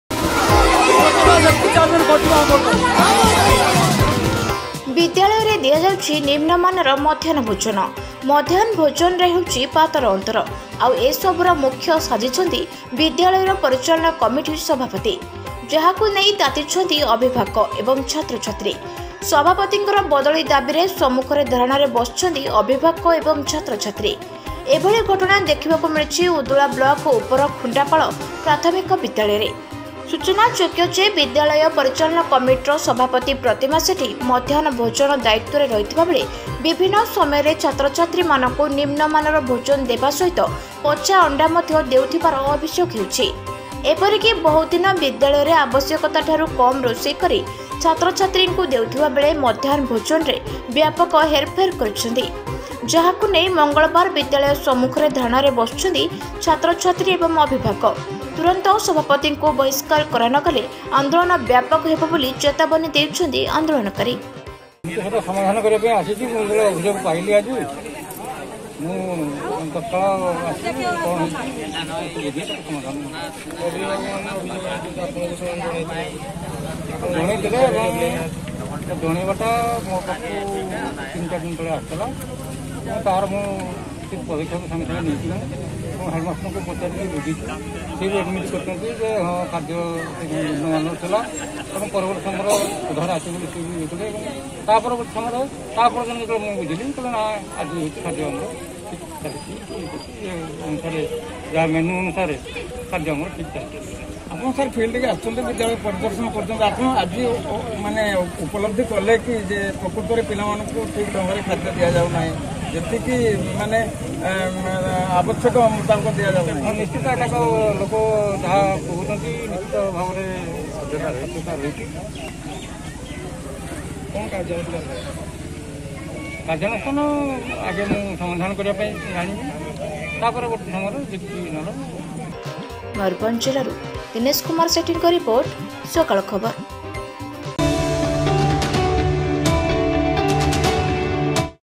ବିଦ୍ୟାଳୟରେ ଦିଆଯାଉଛି ନିମ୍ନମାନର ମଧ୍ୟାହ୍ନ ଭୋଜନ , ଅଭିଭାବକ ଏବଂ ଛାତ୍ରଛାତ୍ରୀଙ୍କ ଧାରଣା
ବିଦ୍ୟାଳୟରେ ଦିଆଯାଉଛି ନିମ୍ନମାନର ମଧ୍ୟାହ୍ନ ଭୋଜନ ,ଚେୟାରମ୍ୟାନଙ୍କ ବଦଳି ଦାବିରେ ବିଦ୍ୟାଳୟ ସମ୍ମୁଖରେ ଅଭିଭାବକ ଏବଂ ଛାତ୍ରଛାତ୍ରୀଙ୍କ ଧାରଣା